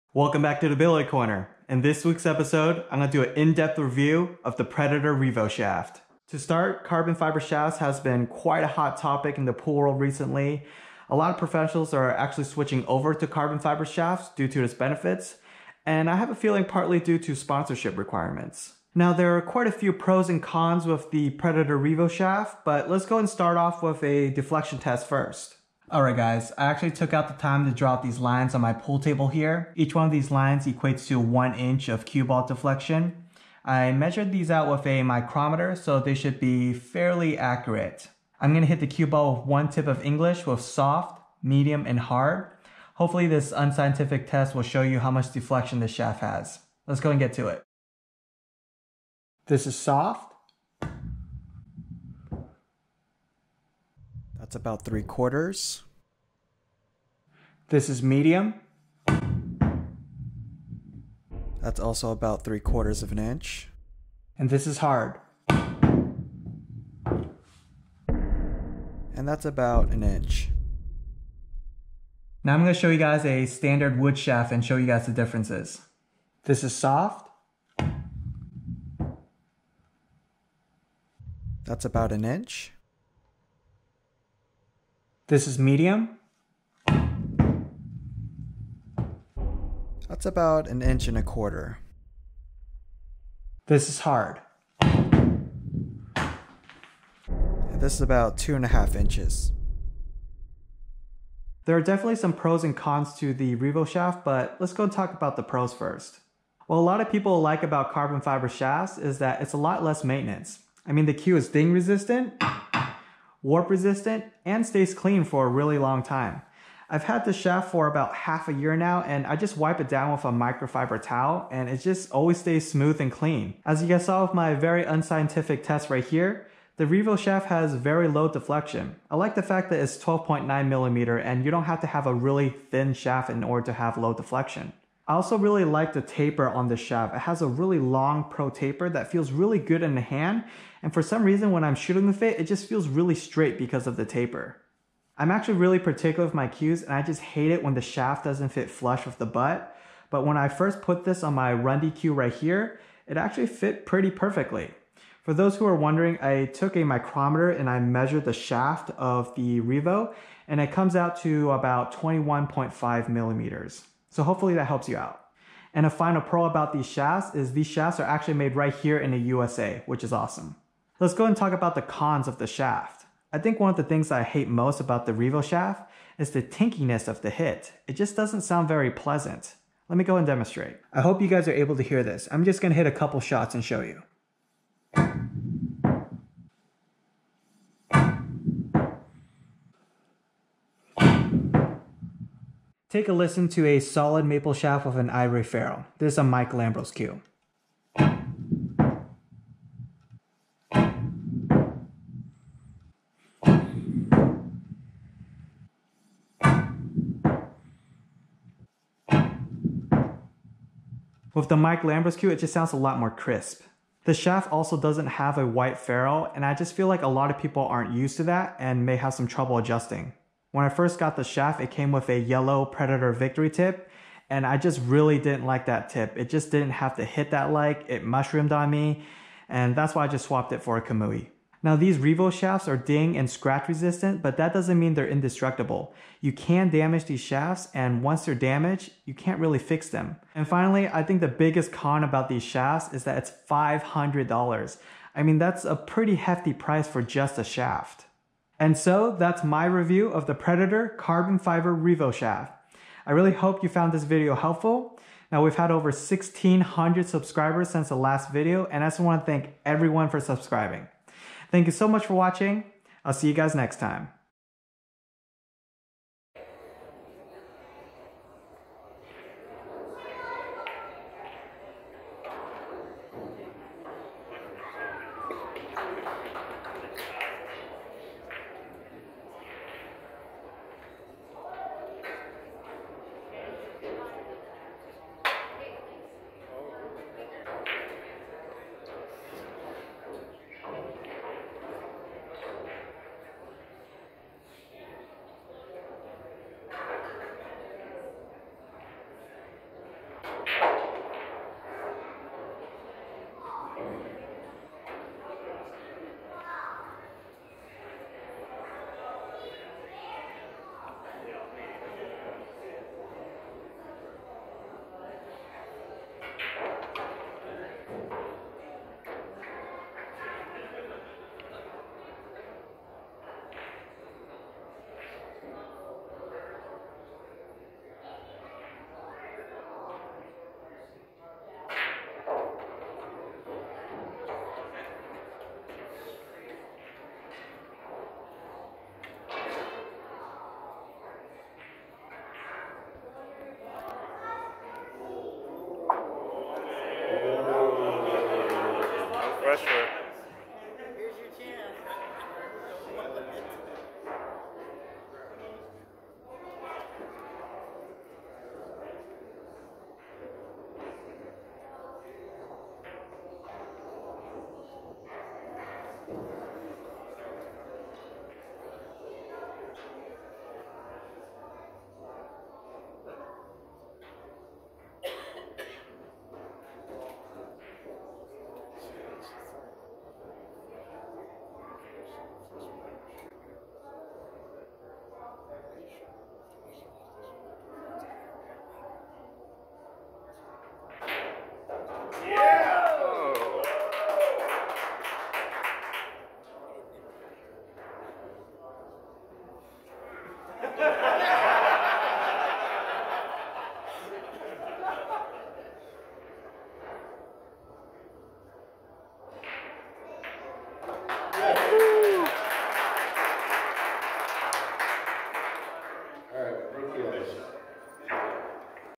In-Depth Predator Revo Shaft Review // Deflection Tests and Comparison
In this weeks episode, I will be doing an in-depth review of the Predator Revo shaft. Is this shaft worth the $500 price tag? That's for you to decide.

Join my Facebook groups to chat with me:
The Billiard Corner:

0:00-0:47 - Introduction
0:48-1:40 - Deflection Test
1:41-2:59 Pros & Cons of Carbon Fiber Shafts
3:00-3:33 - Sound Test
3:34-4:08 - Cons Of Revo
4:09-4:25 - Conclusion
4:26-6:58 - Bonus Clip